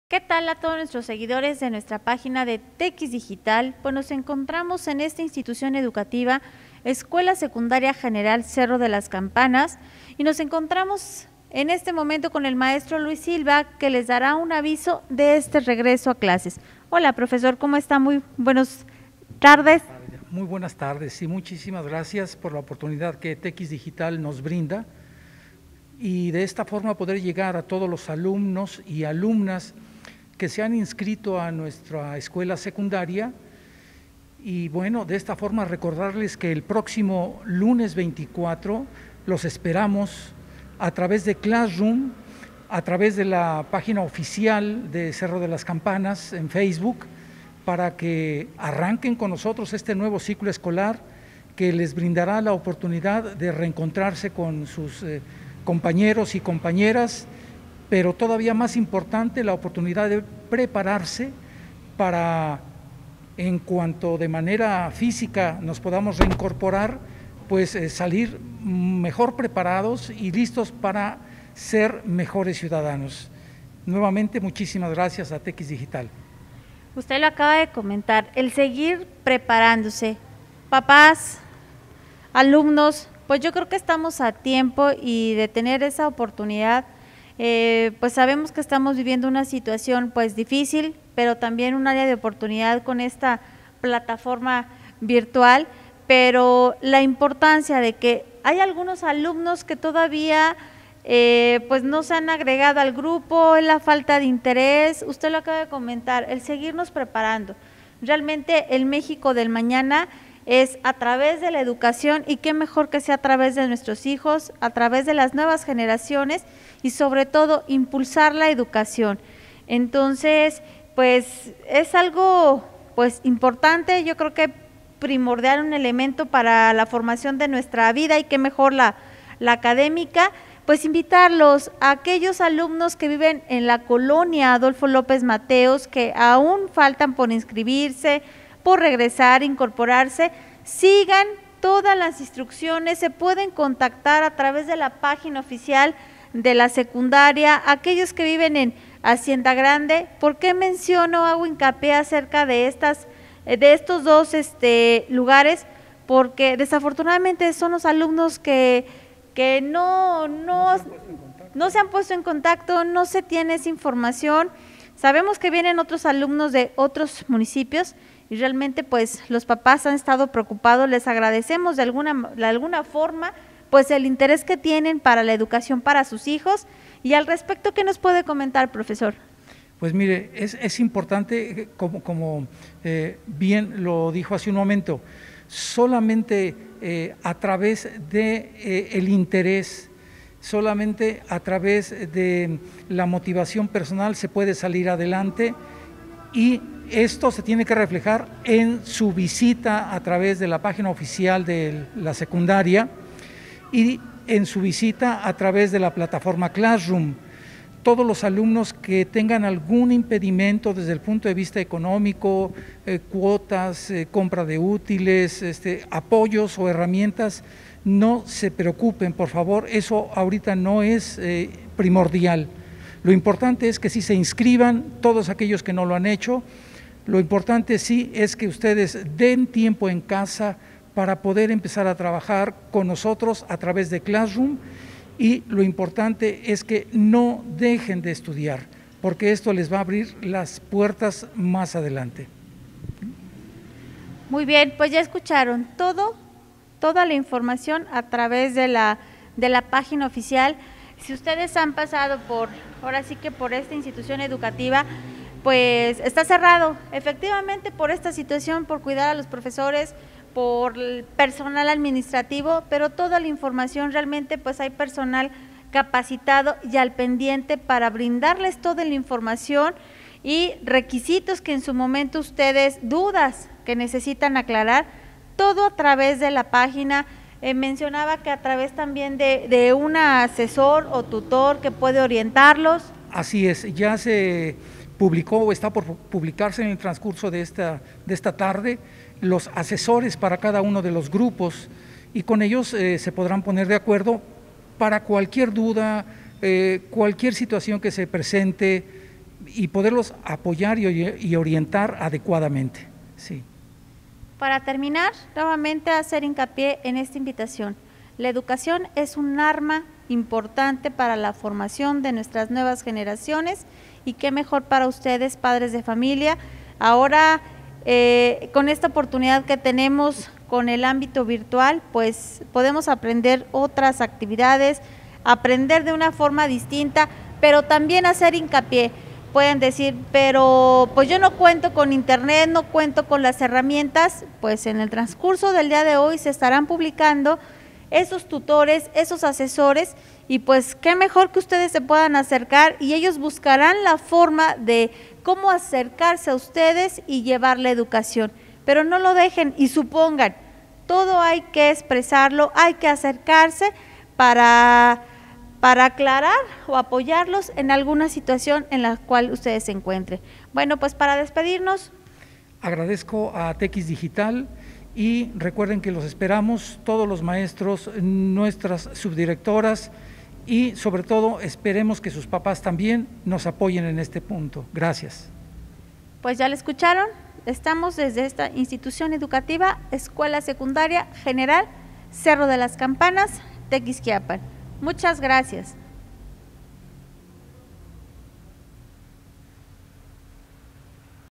ESC. SEC. GRAL. CERRO DE LAS CAMPANAS - Tkiss Digital
Invitación a los padres de familia a inscribir a sus hijos en la Escuela Secundaria General Cerro de las Campanas.